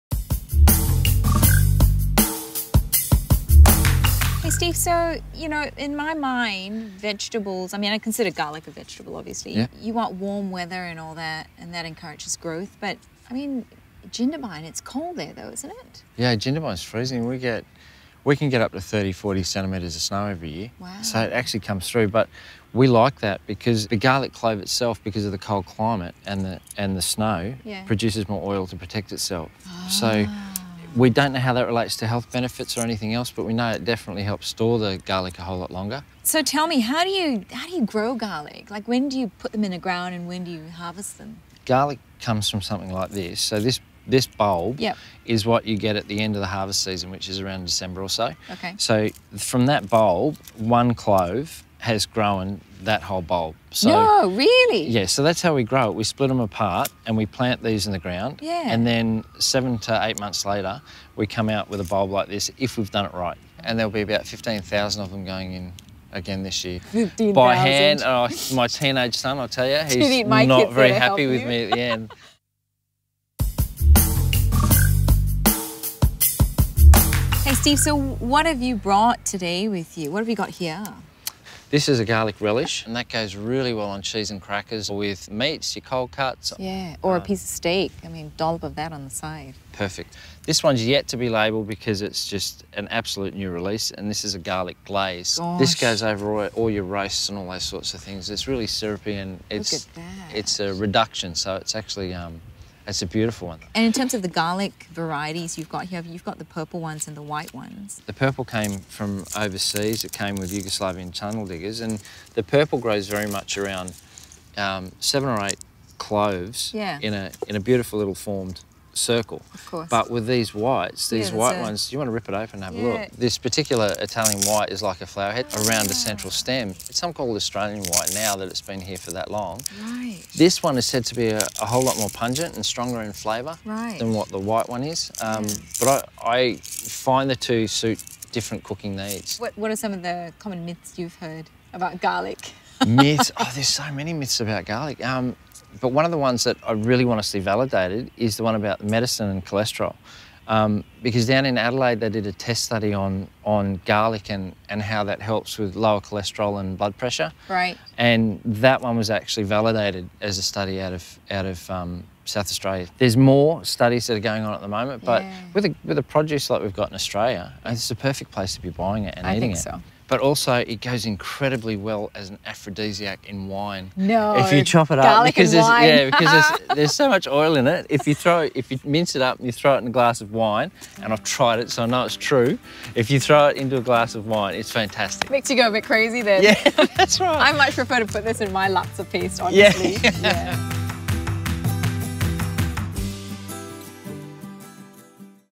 Steve Wallis, Snowy Mountains Garlic
Audra chats to Steve about the facts and myths surrounding garlic, including a good tip for use as an aphrodisiac.